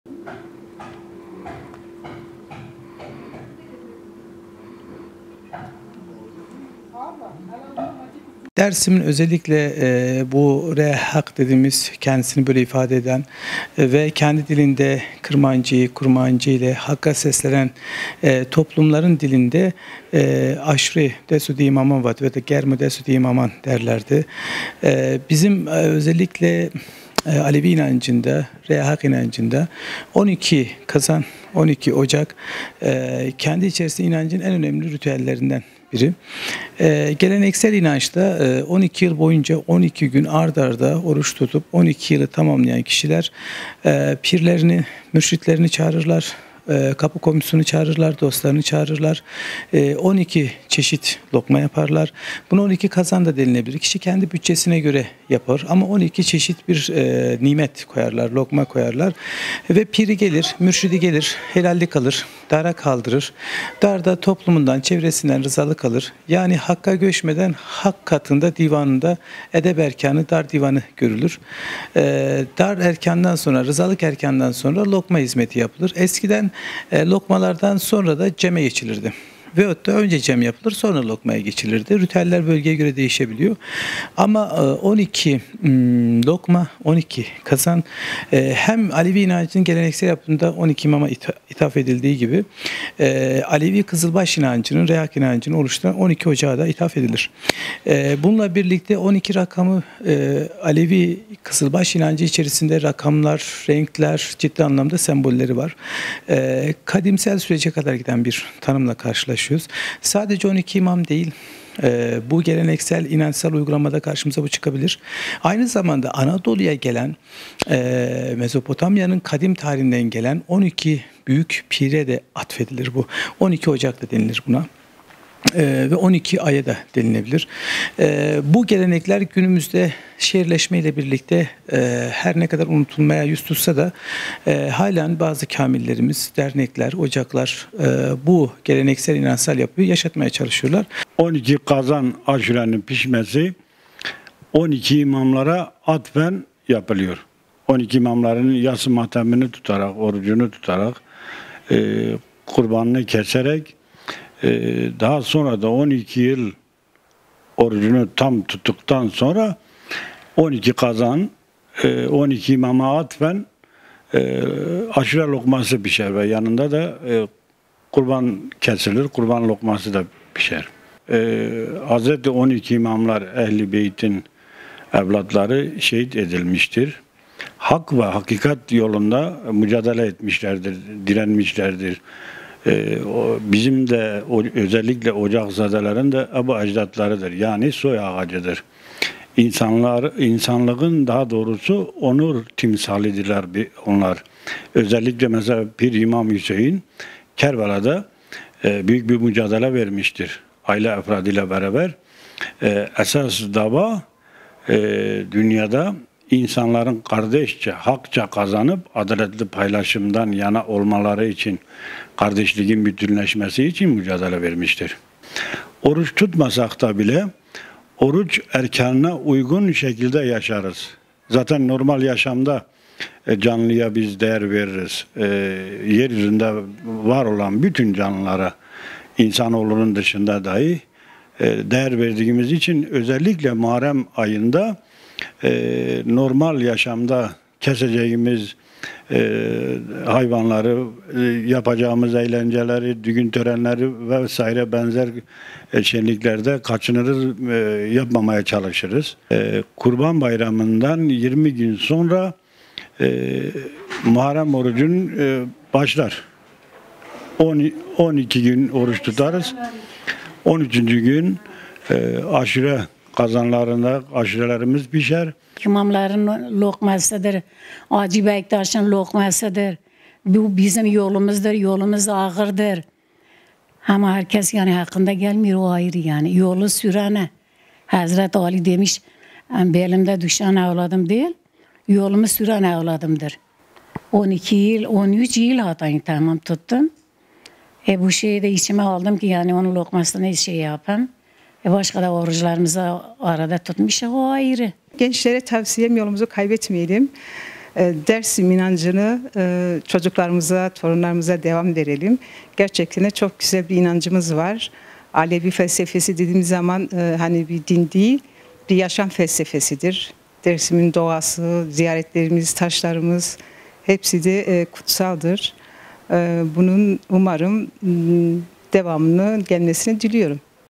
12 yıl Muharrem Orucu tutan Besi Kaldı, 12 kazan kurdu: İnancımızı yaşatıyoruz
PİRHA-12 yıl Muharrem Orucu tutan Ağuçan Ocağı evladı Besi Kaldı, Şahinkaya Cemevi'nde 12 kazan kaynattı. 12 kazanın 12 imamların ve Hacı Bektaş Veli'nin lokması olduğunu belirten Besi Kaldı, "Bizim yolumuz ağırdır. 12 yıl boyunca oruç tuttum ve onun lokmasını pay etmek istedim" dedi.